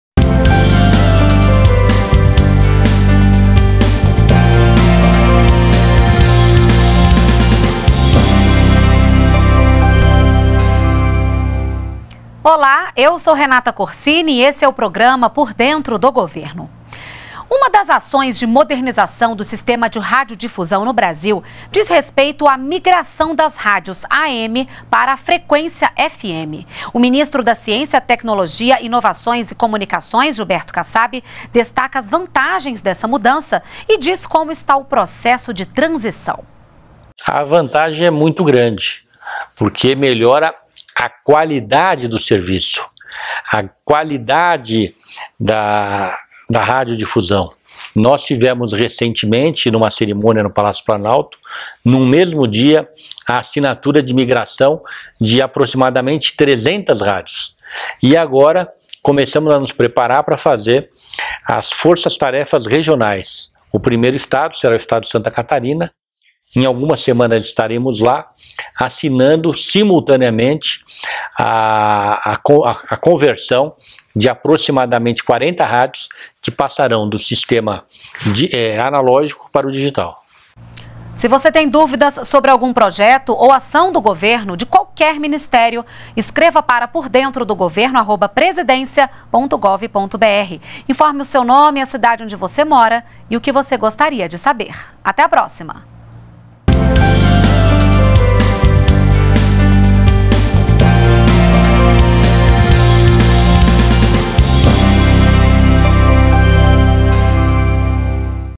Gilberto Kassab explica processo de transição das rádios AM/FM
POR DENTRO DO GOVERNO - 13.01.17: Uma das ações de modernização do sistema de radiodifusão no Brasil diz respeito a migração das rádios AM para a frequência FM. O ministro da Ciência e Tecnologia destaca as vantagens dessa mudança e diz como está o processo da transição.